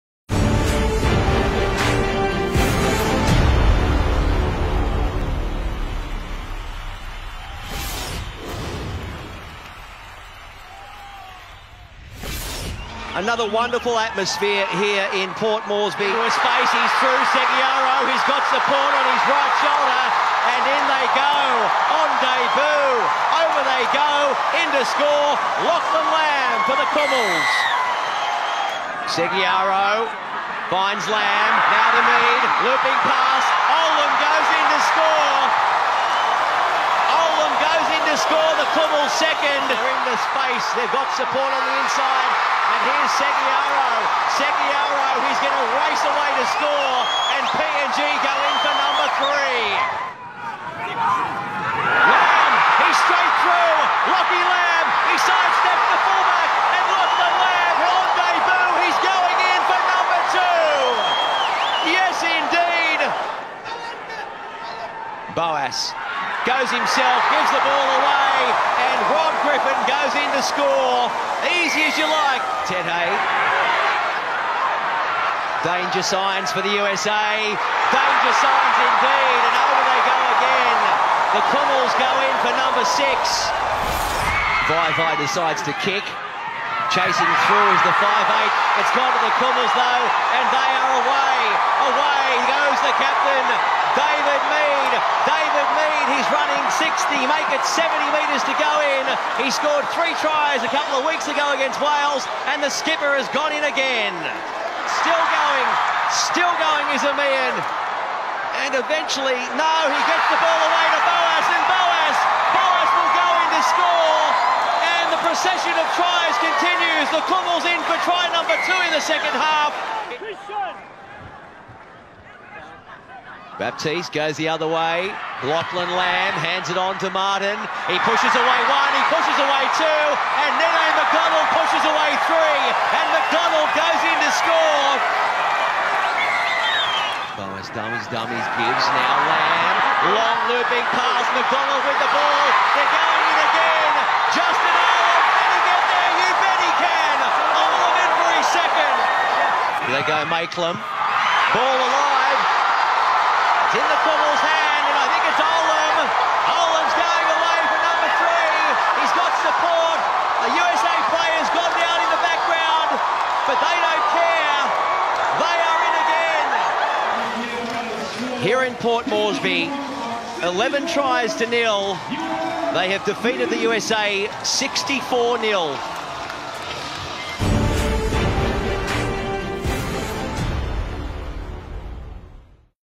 2017 Rugby League World Cup: PNG v USA
Highlights from the 2017 Rugby League World Cup clash between the Papua New Guinea Kumuls and USA Hawks